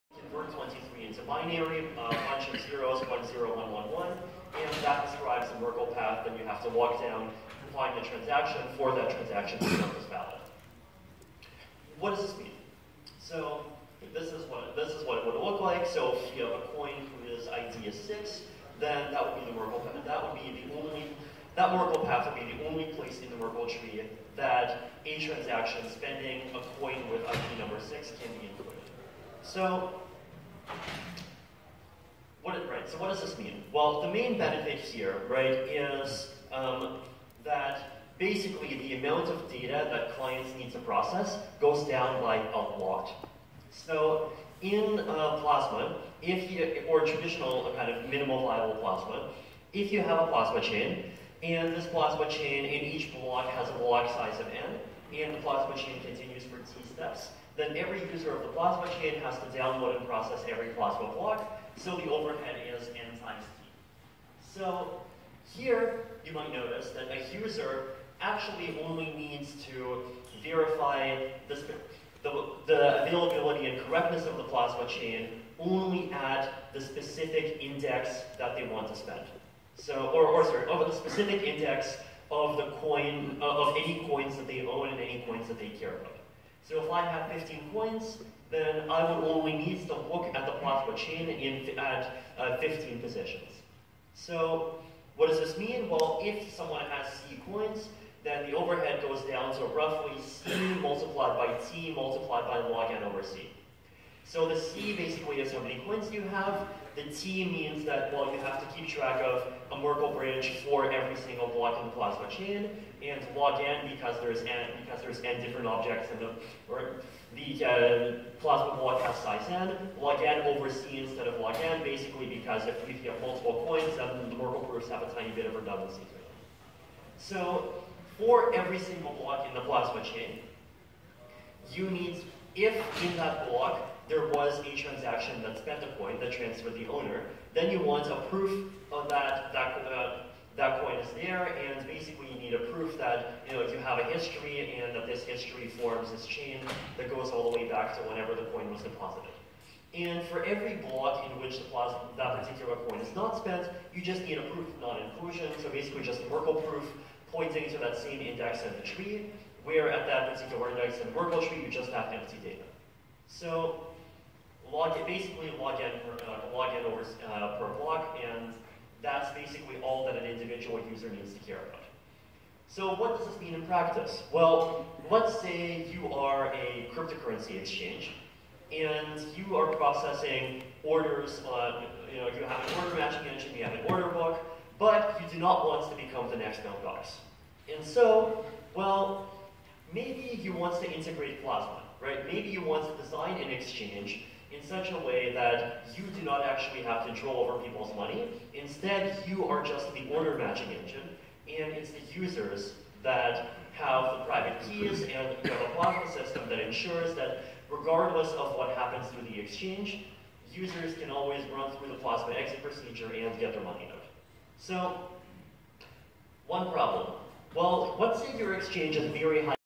Vitalik Buterin Plasma Cash ETHcc 2018 Paris (1)
Vitalik Buterin - "A quick intro to Plasma cash" presentation within the ETHCC 2018 conference, Paris. The functioning of the new Merkle trees 1.